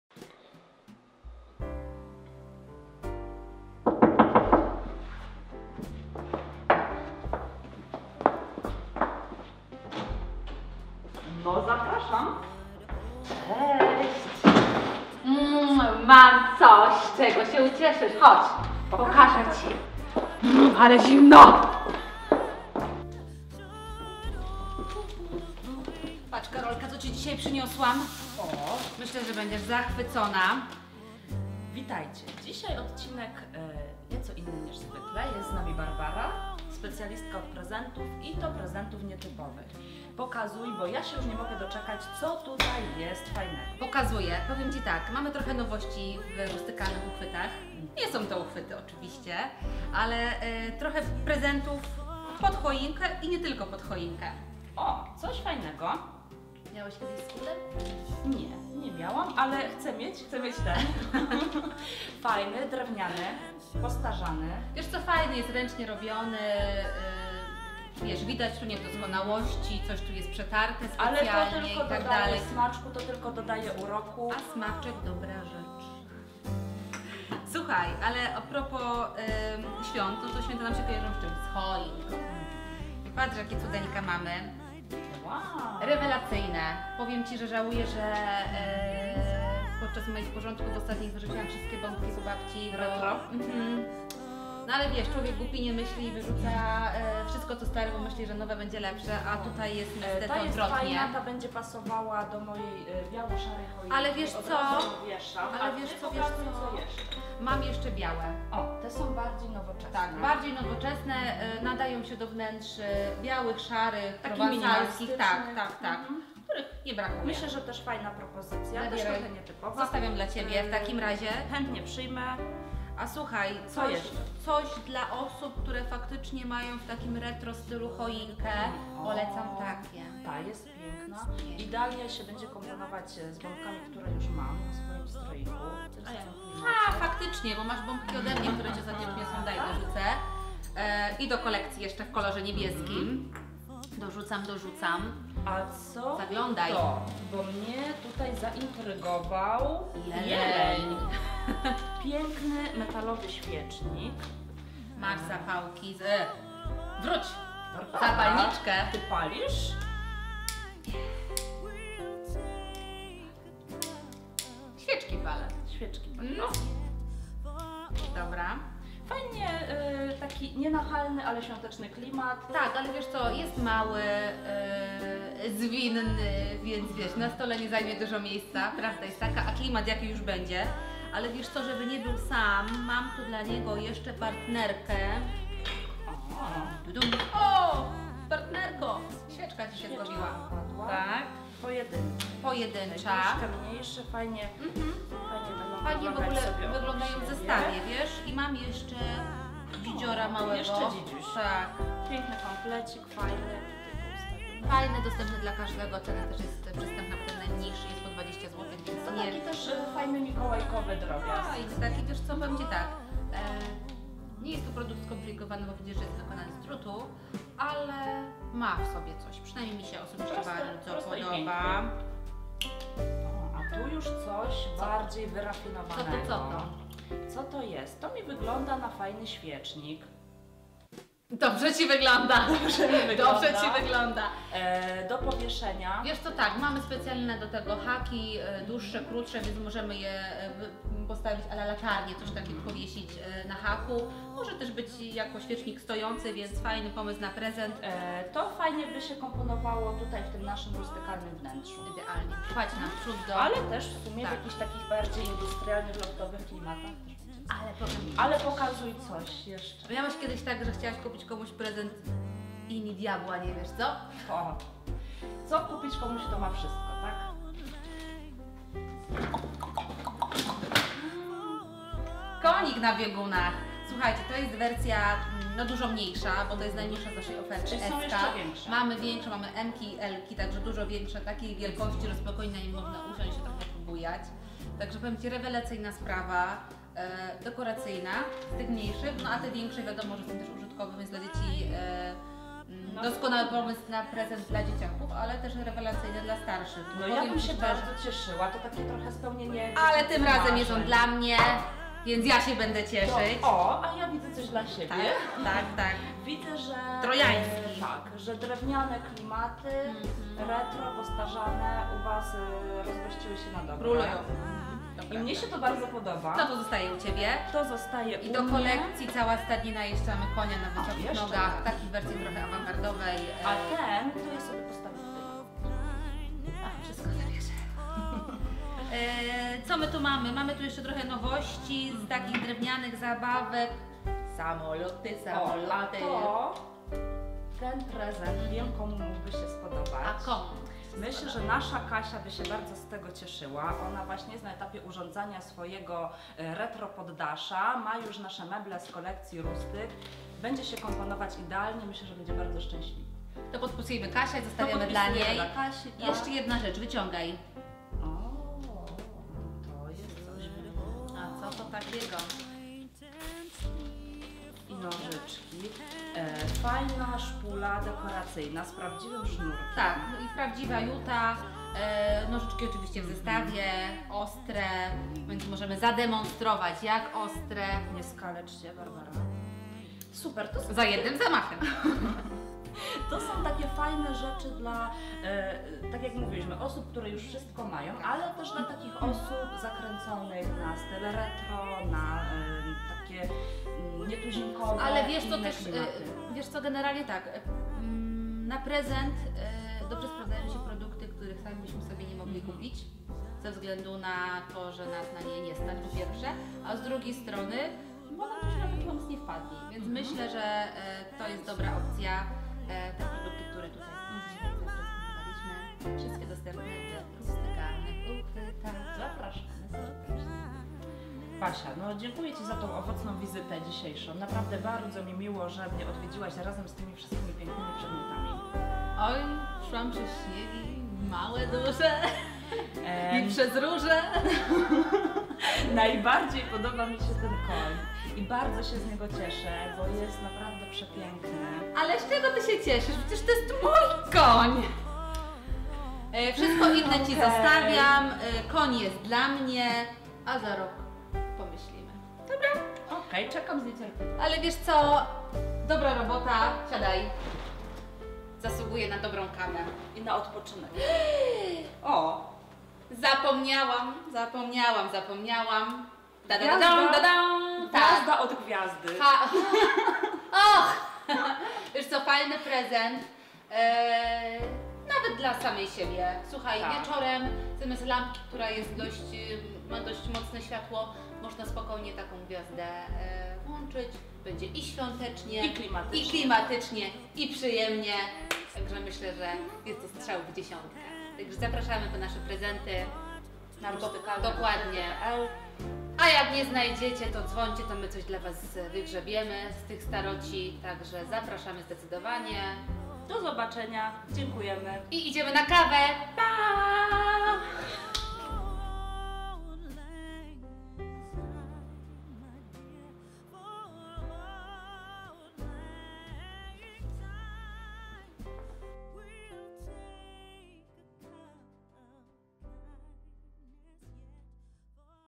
Pomysł na prezent - nietypowy, wyjątkowy i stylowy
Jak wybrać prezent dla kogoś, kto ma już wszystko? Co kupić komuś, kto lubi niebanalne rzeczy? Co będzie idealnym prezentem dla fanów stylu retro? Oglądajcie!